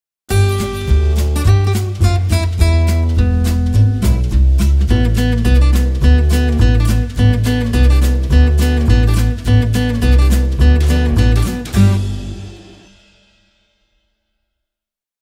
Abalone by Kyser 3
Kyser's new ABALONE capo is our classiest look yet. You'll love the iridescent tones and luminous colors that transform from every angle.